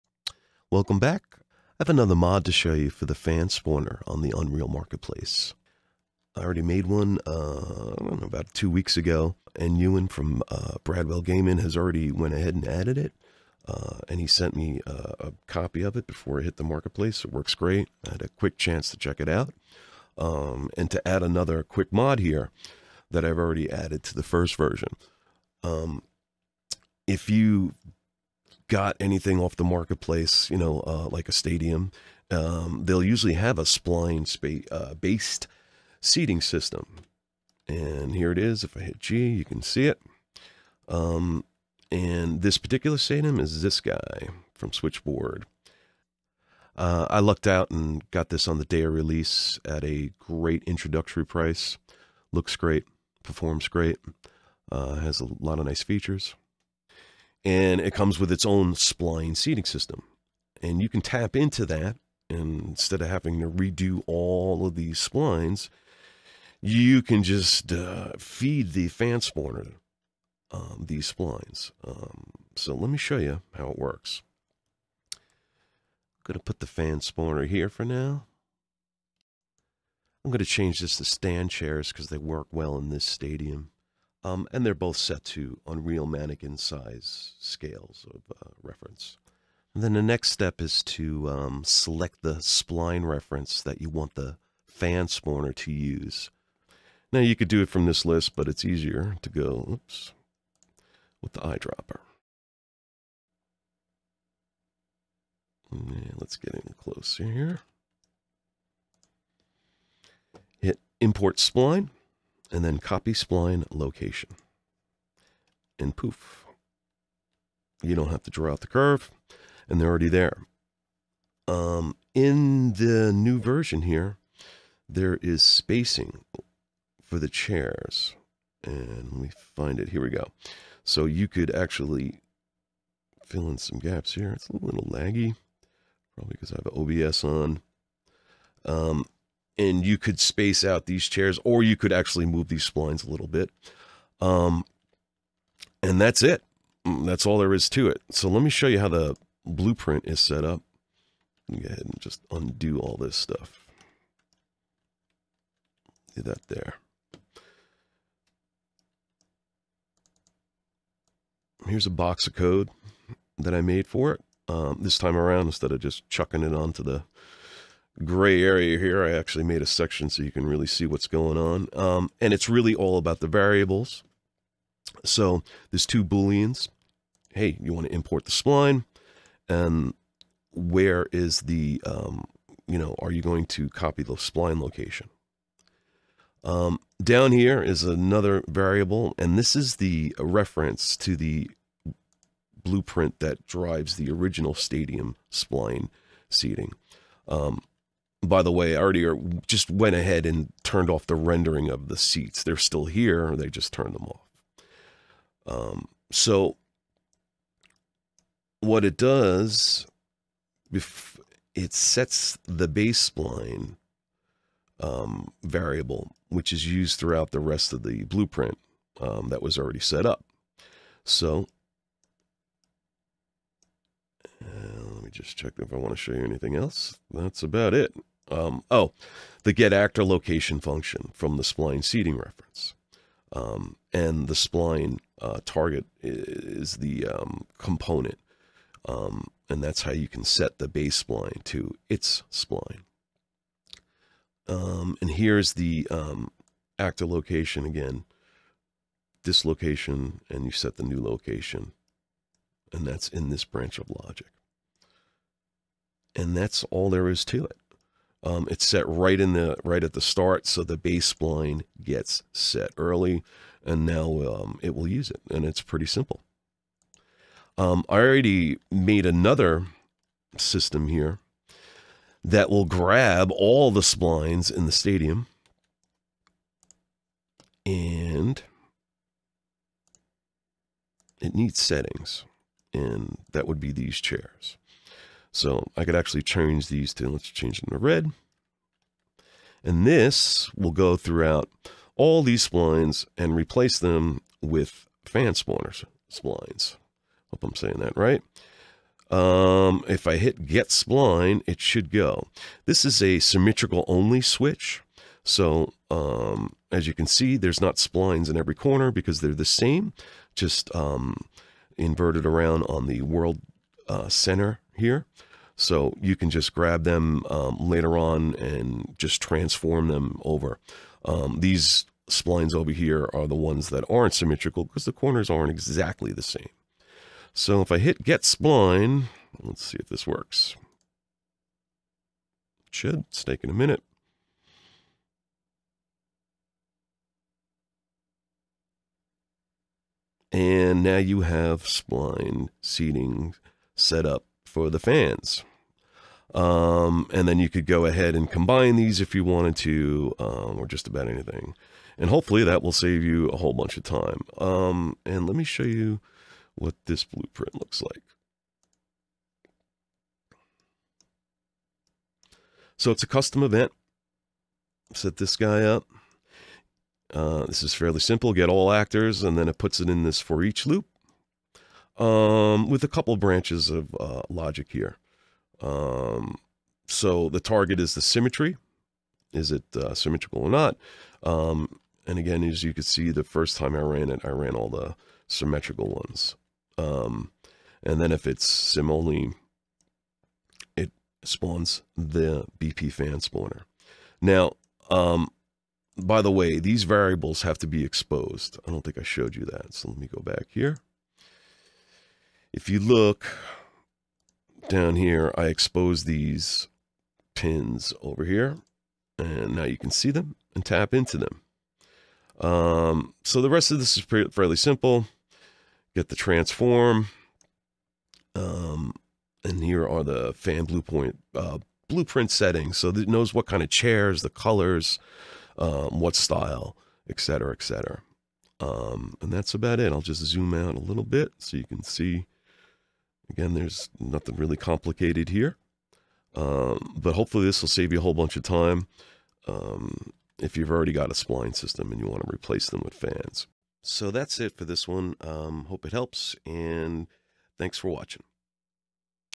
Unreal Crowd Plugin Spline Import Mod
A quick tutorial on importing existing spline seating for the Line editor function in the Fan Spawner blueprint.
00:00 Intro and Demo
02:40 Fan Spawner Blueprint Mod Overview
04:26 Spline Batch Import Intro and Demo
05:57 Spline Batch Import Blueprint